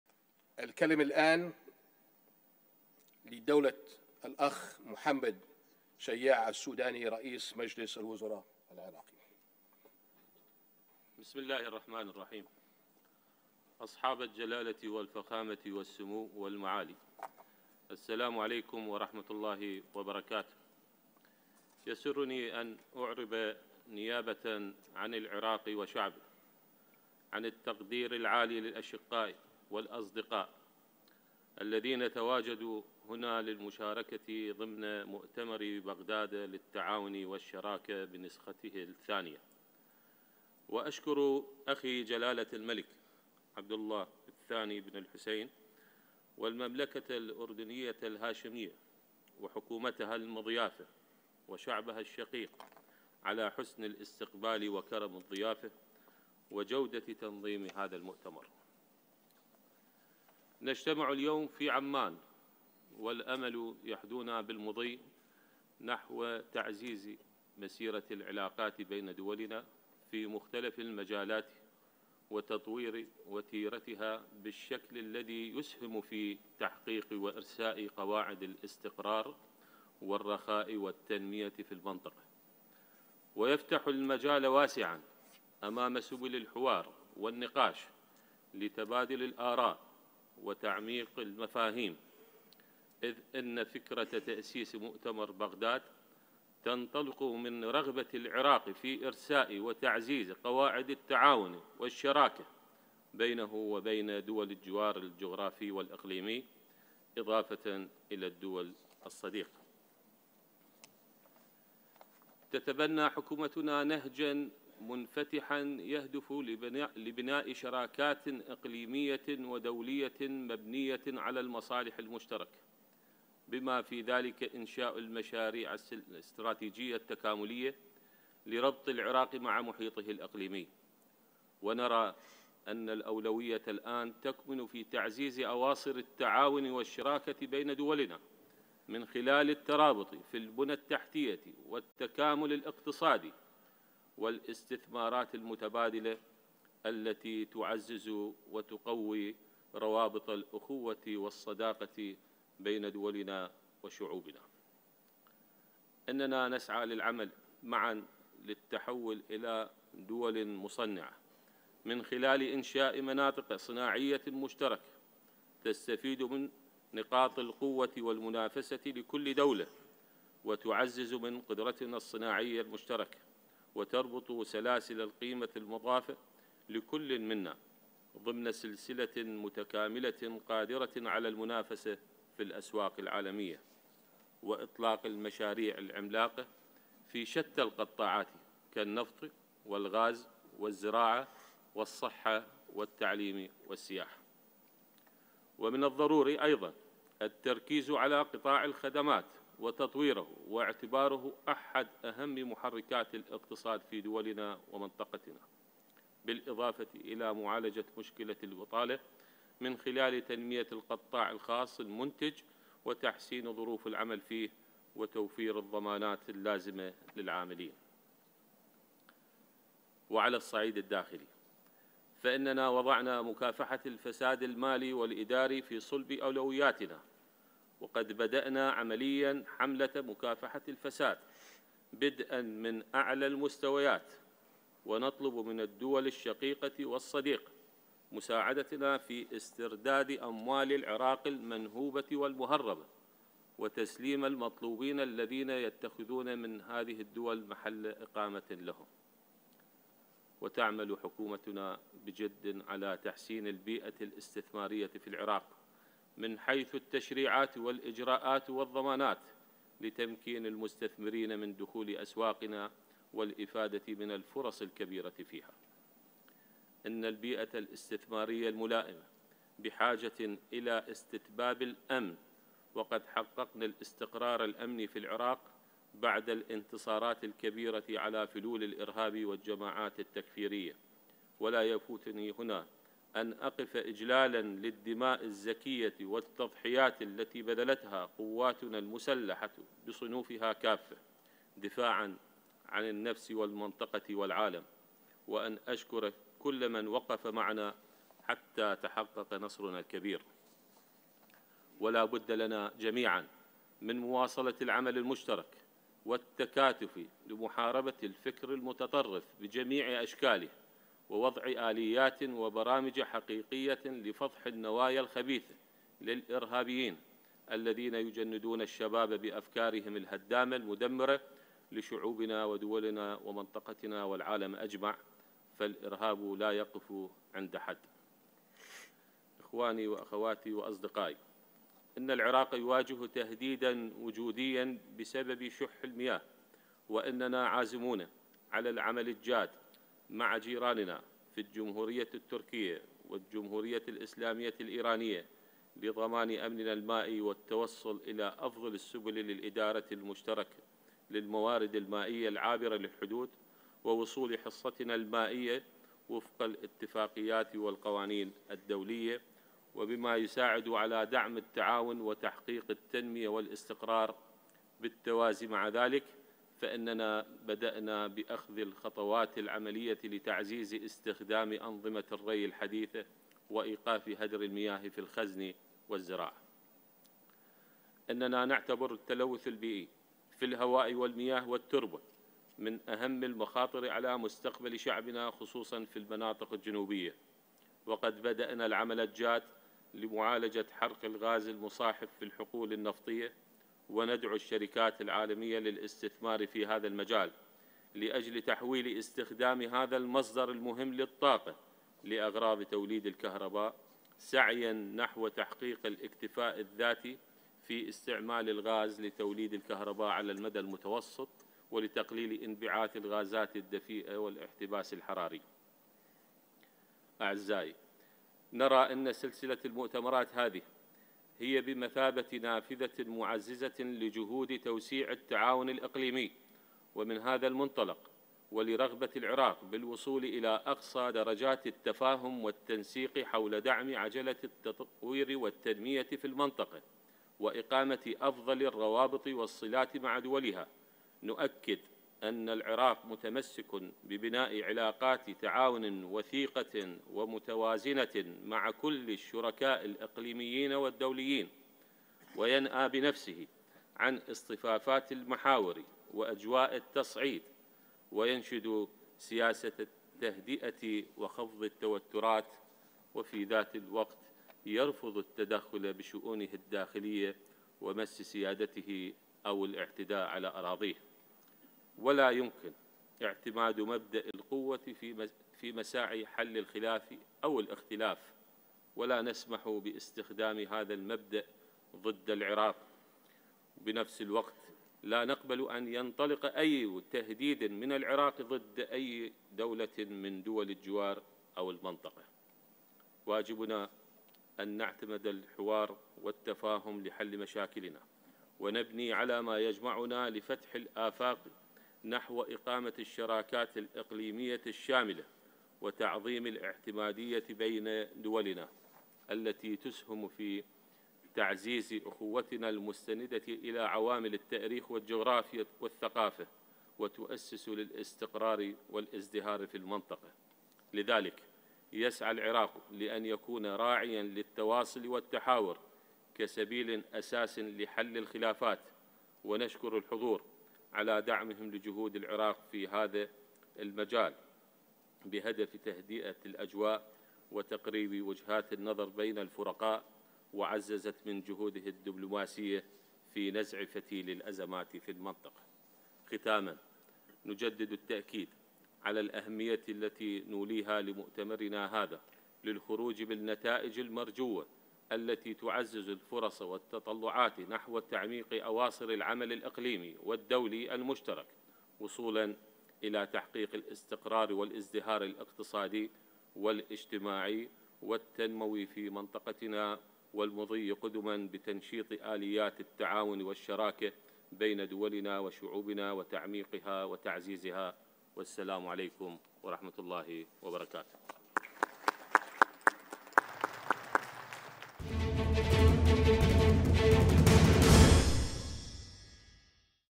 شاهد .. كلمة هامة لـ السوادني بـ مؤتمر بغداد الثاني في الاردن
كلمة رئيس الوزراء العراقي محمد شياع السوداني في مؤتمر بغداد الثاني للتعاون والشراكة بالأردن
السوداني: العراق يرفض التدخل في شؤونه الداخلية والاعتداء على أراضيه
السوداني: العراق يواجه تهديدا وجوديا بسبب شح المياه وعازمون على معالجة ذلك
السوداني: نسعى للعمل معا للتحول من دول مستهلكة الى مصنعة وإنشاء مناطق صناعية
السوداني: نطلب من الدول الشقيقة والصديقة مساعدتنا في استعادة الأموال المهربة
السوداني: وضعنا مكافحة الفساد والمالي والإداري في صلب أولوياتنا
كلمة رئيس الوزراء في مؤتمر بغداد الثاني للتعاون والشراكة بالأردن
شاهد .. كلمة السوادني بـ مؤتمر بغداد الثاني  في الاردن

موقع قناة التغيير الفضائية الالكتروني